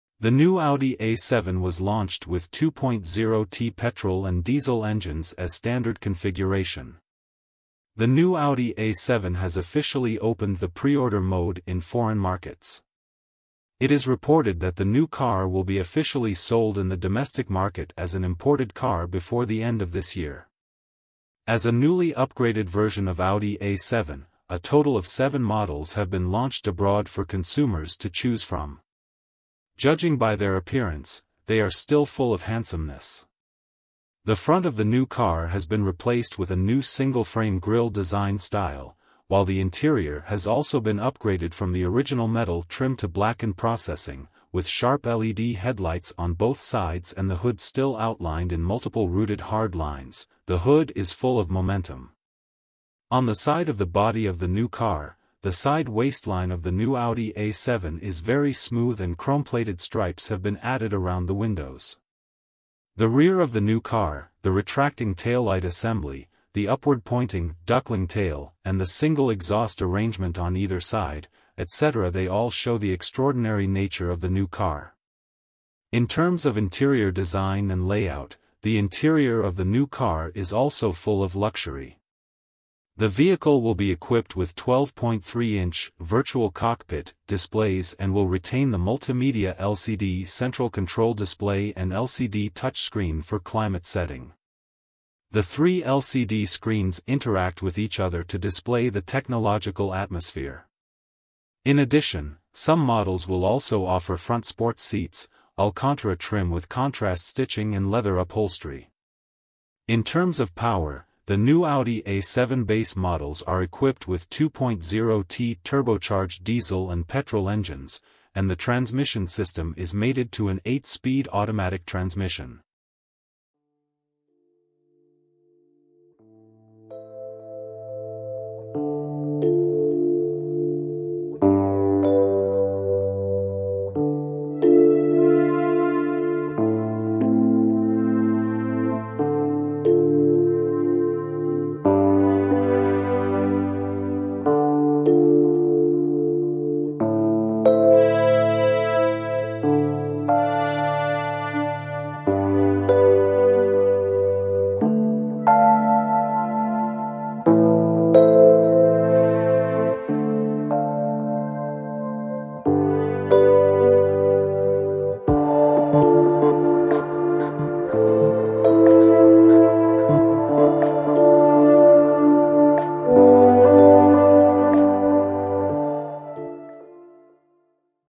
Launched with 2.0T Gasoline and Diesel engine as Standard Configuration, New Audi A7 2023-2024
The new Audi A7 has officially opened the pre-order mode in foreign markets. It is reported that the new car will be officially sold in the domestic market as an imported car before the end of this year.

  Price Range : 585,800-784,800 Yuan

As a newly upgraded version of Audi A7, a total of 7 models have been launched abroad for consumers to choose from. Judging by their appearance, they are still full of handsomeness. The front of the new car has been replaced with a new Singleframe grille design style, while the interior has also been upgraded from the original metal trim to blackened processing, with sharp LED headlights on both sides and the hood still outlined in multiple Rooted hard lines, the hood is full of momentum.

On the side of the body of the new car, the side waistline of the new Audi A7 is very smooth and chrome-plated stripes have been added around the windows. The rear of the new car, the retracting taillight assembly, the upward-pointing "duckling tail" and the single exhaust arrangement on either side, etc. they all show the extraordinary nature of the new car.

In terms of interior design and layout, the interior of the new car is also full of luxury. The vehicle will be equipped with 12.3-inch "virtual cockpit" displays and will retain the multimedia LCD central control display and LCD touchscreen for climate setting. The three LCD screens interact with each other to display the technological atmosphere. In addition, some models will also offer front sports seats, Alcantara trim with contrast stitching and leather upholstery.

In terms of power, the new Audi A7 base models are equipped with 2.0T turbocharged diesel and petrol engines, and the transmission system is mated to an 8-speed automatic transmission.